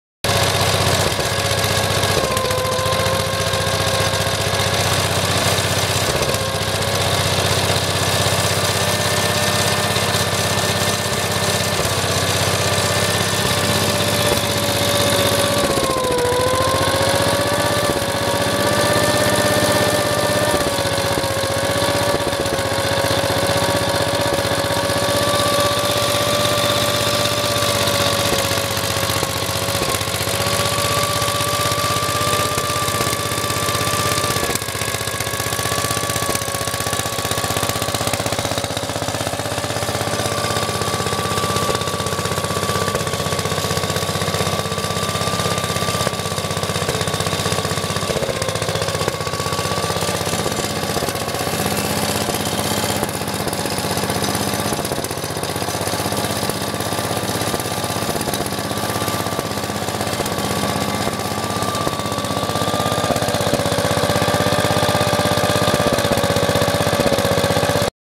Howard Gem rotary hoe with British Junior engine
This is a Howard Gem rotary hoe dating from 1953.  It is fitted with a British Junior 660cc sv engine, built in house.  It has three speed and reverse gearbox.  This example in very original condition but needs an original muffler.